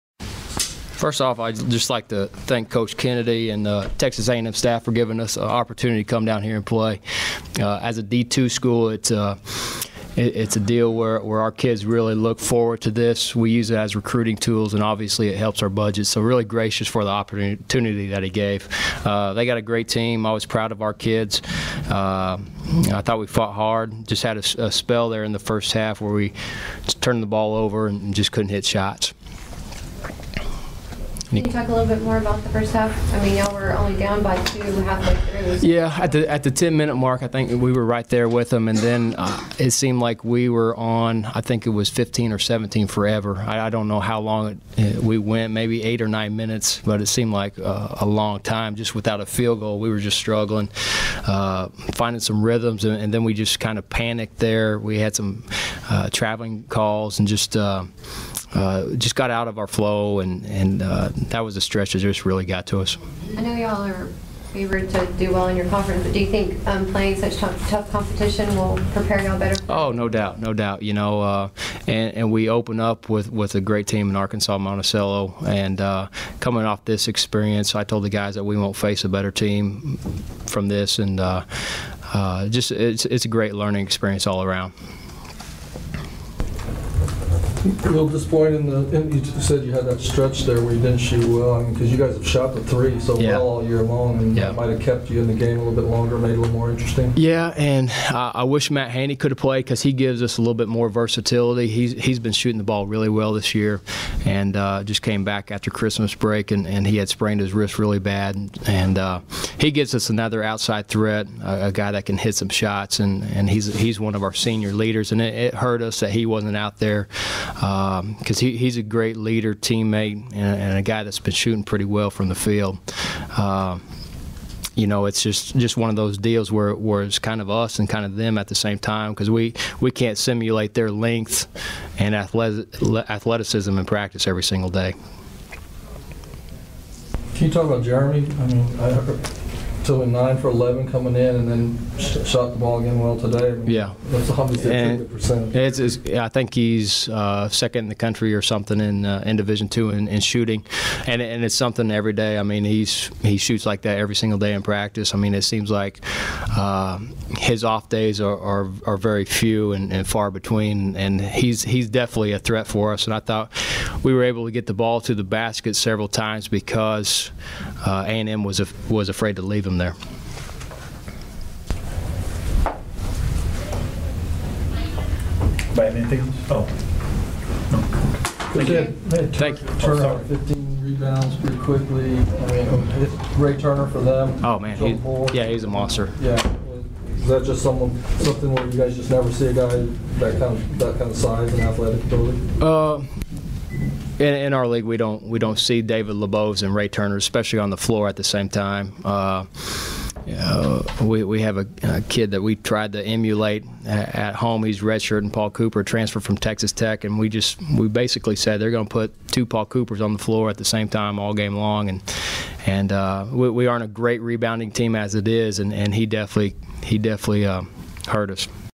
Aggie MBB: Ark. Tech Coach Karleskint (Ark. Tech Post-Game)
Arkansas Tech University Wonderboys' Basketball Head Coach Doug Karleskint met with the media following the loss to Texas A&M on December 29th, 2011.